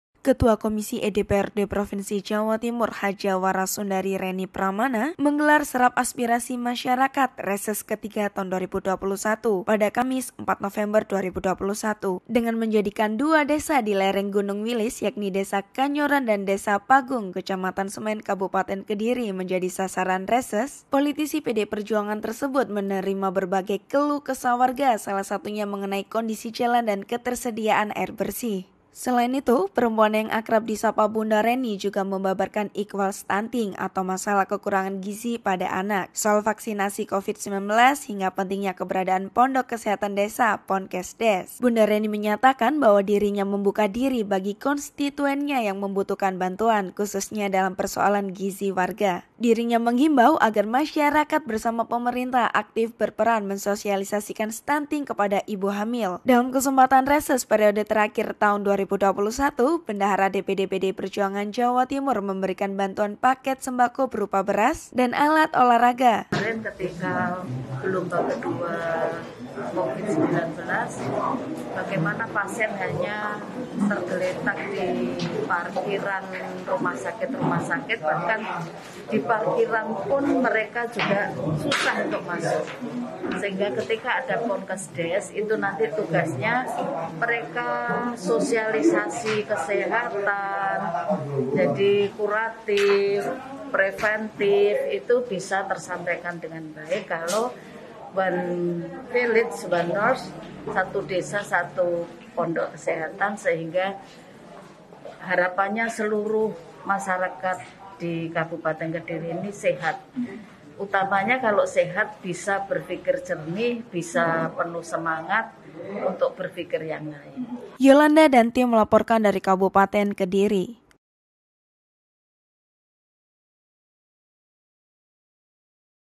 Reses di Lereng Wilis, Renny Pramana Serap Aspirasi Sambil Edukasi Soal Stunting
Reses Ketua Komisi E DPRD Provisinsi Jawa Timur.

Dua desa di Lereng Gunung Wilis jadi sasaran reses Ketua Komisi E DPRD Provinsi Jawa Timur, Wara Sundari Renny Pramana. Yakni Desa Kanyoran dan Desa Pagung, Kecamatan Semen, Kabupaten Kediri.